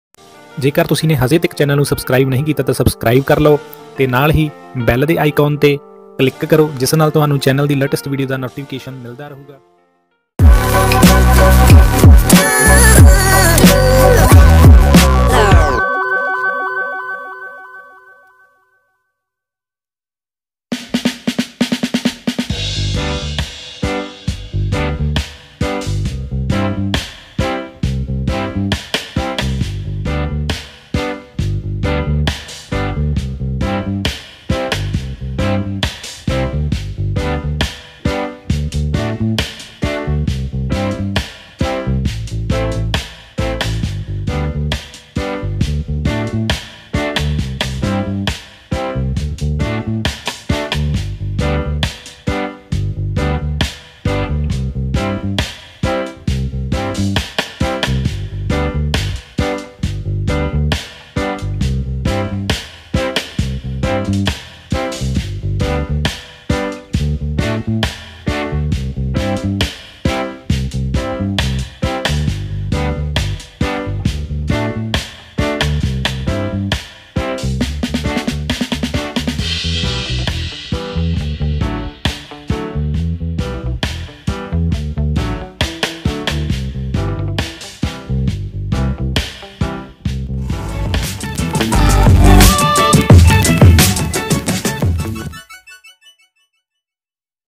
Mahindra Arjun 65 4×4 tractor looks like Luxury Red Ferrari Cars ।
Hello dosto, Iss Video Mein aap dekho ge. Mahindra Arjun Novo 65 4×4 tractor ko. Jo k Mahindra and Company ne new lauch kiya hai. Mujhe Umeed hai ki apko Video pasand ayegi.